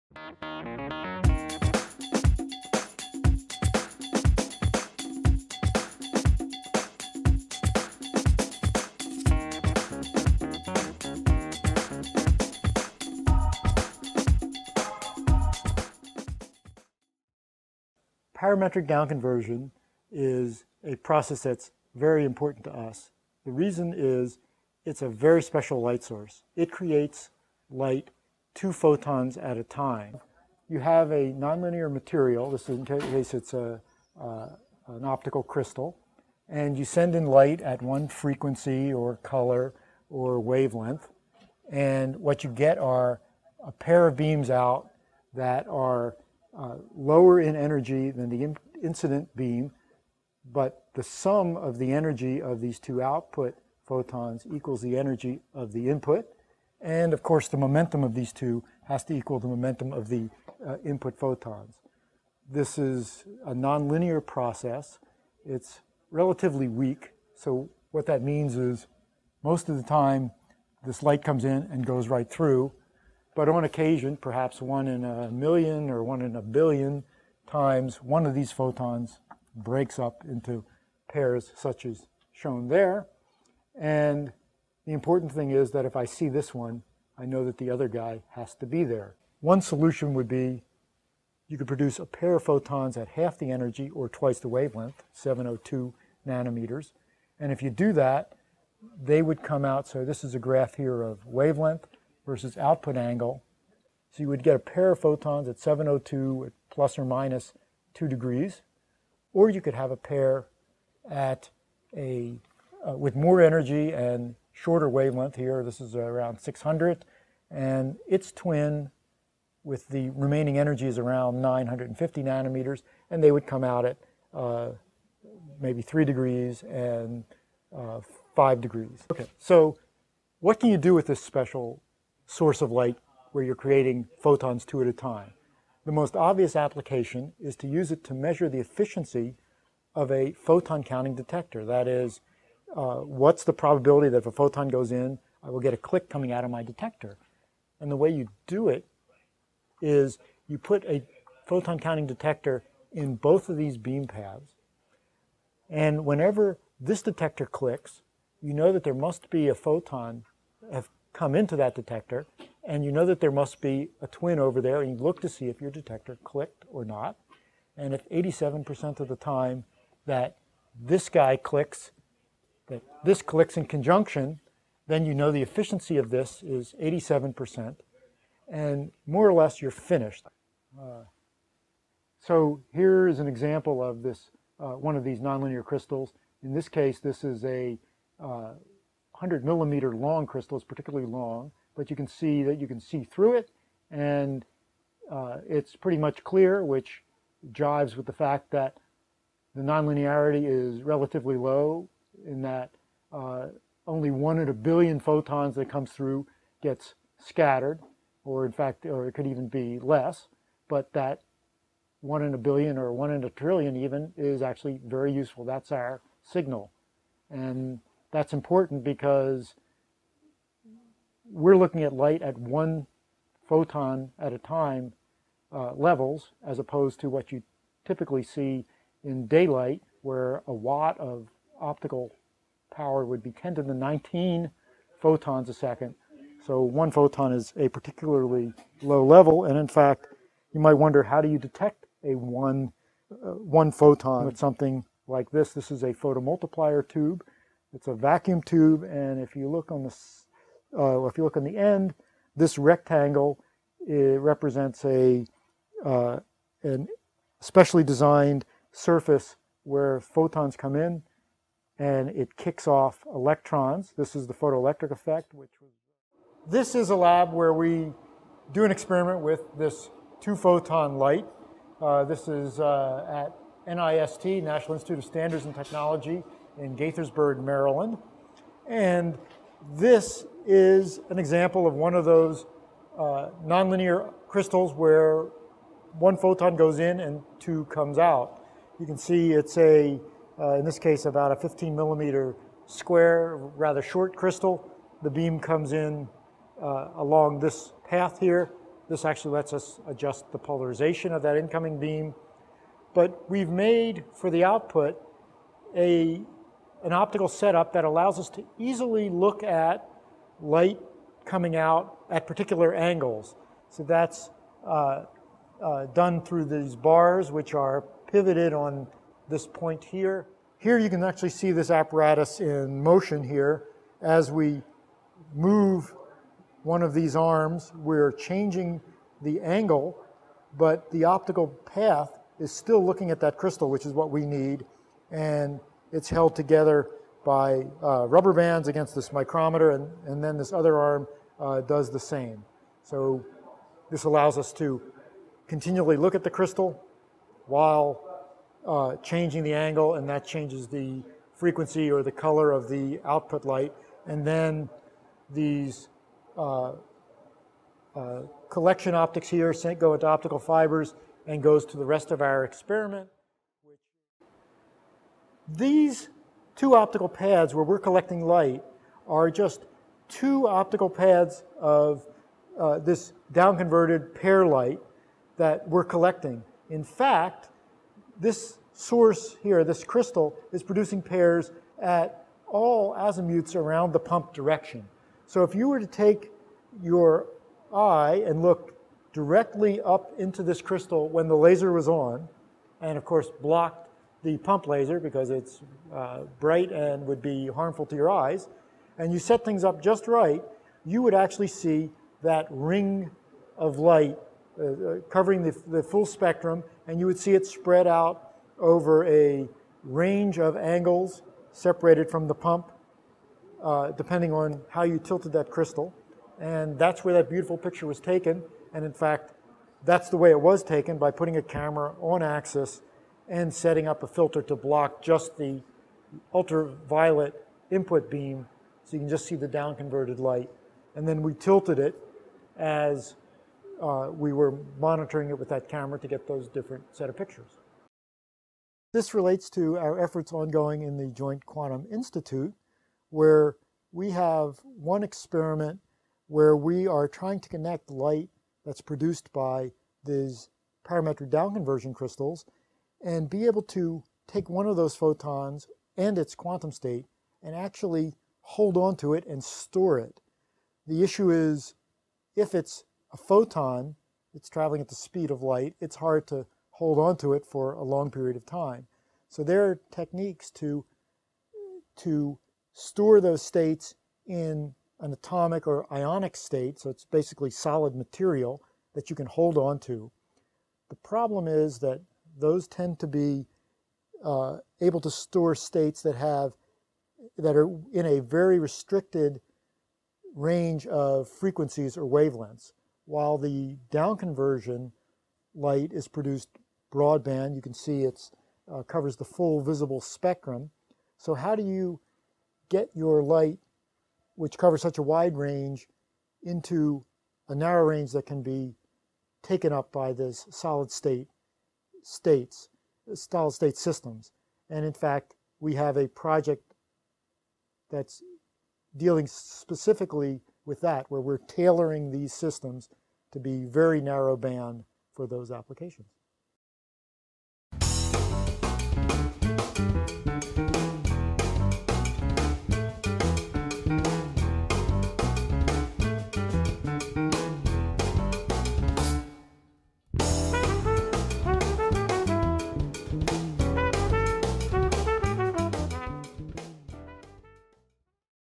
One Photon In, TWO Photons Out
A fascinating phenomenon with a forbidding name (parametric down conversion) occurs in certain crystals. A single photon enters, but two emerge. There are myriad pair combinations; but in each case, the sum of the total energy and momentum of the two output photons exactly equals those of the single input photon.

Parametric down conversion is finding a host of uses in physics, from testing the efficiency of high-tech single-photon detectors to cutting-edge experiments in quantum mechanics.

Alan Migdall of the National Institute of Standards and Technology explains and demonstrates how it's done.